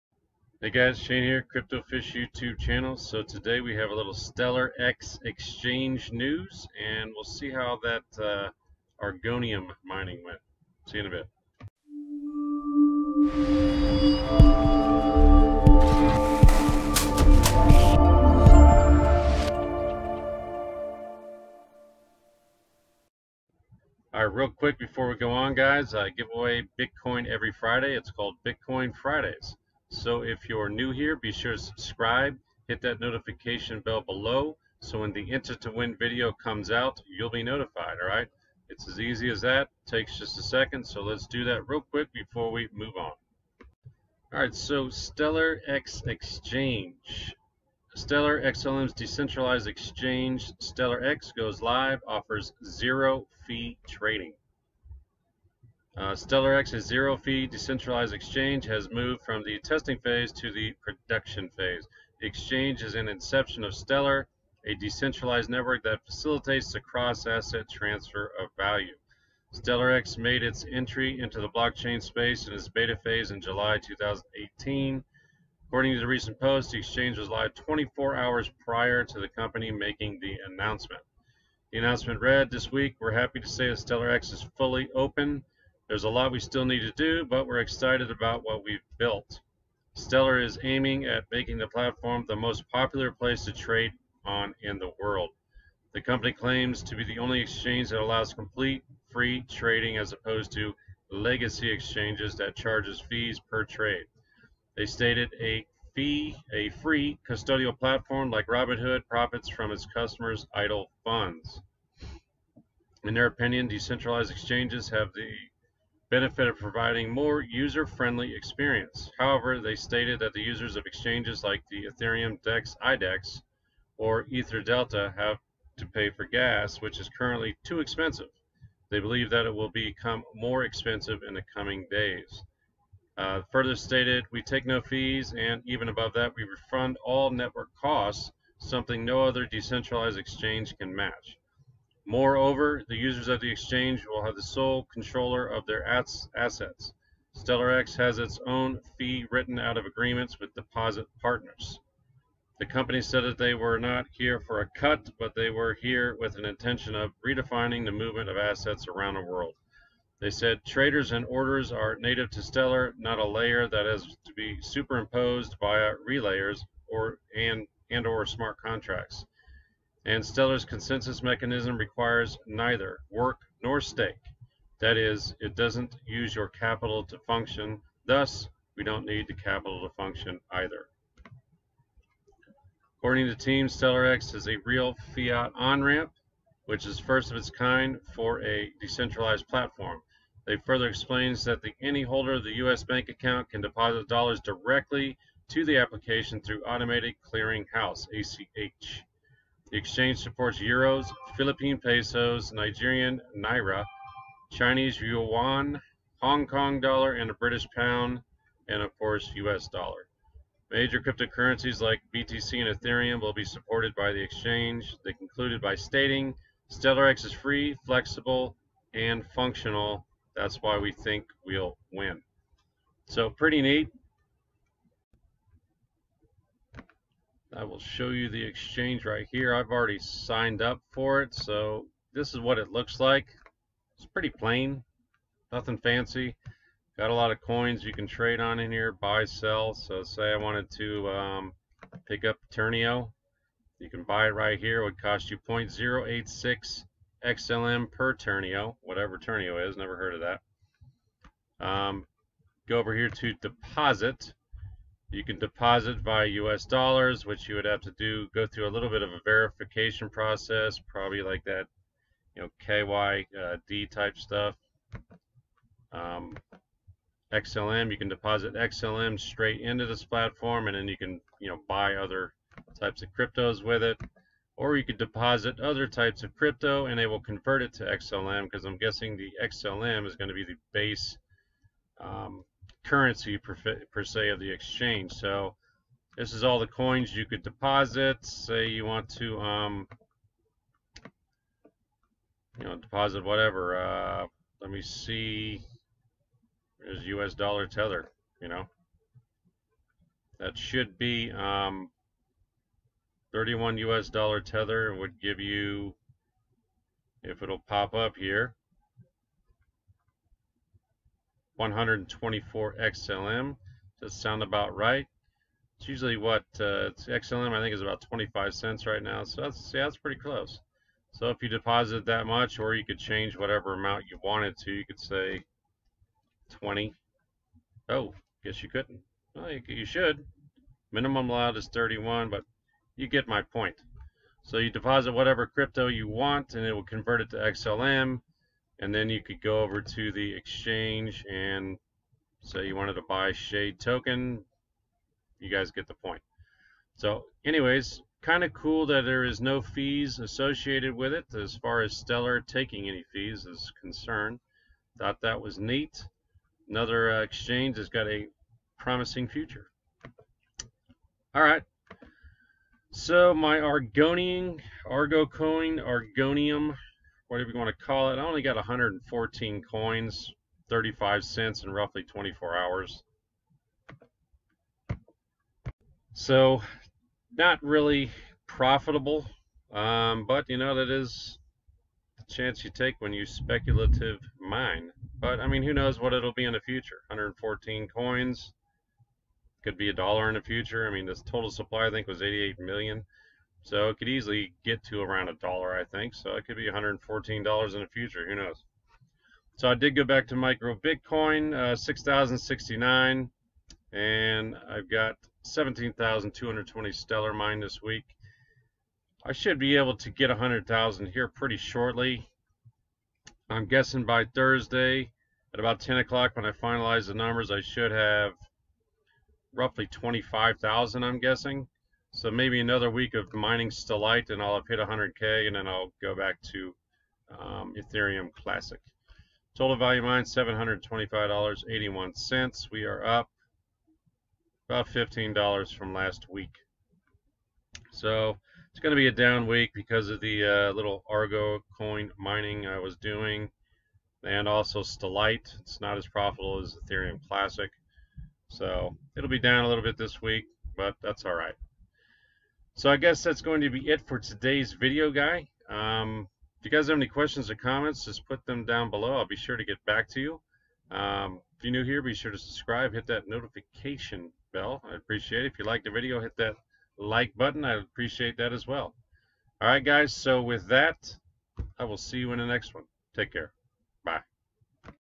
Stellar X Exchange Opens With Zero Fee Trades!
Stellar is at it again, this time they have officially launched their Stellar X Exchange.  The Stellar X decentralized Exchange will offer zero fee trades, for life.  The exchange is in it's early beginnings with a lot more to do but it is live and operational.  They also offer a few Fiat to crypto tethers too make trading from markets like Nigeria accessible.  The xlm news hasn't seemed to boost the price at this point.  This is more Stellar lumens news about a project by the Stellar XLM team.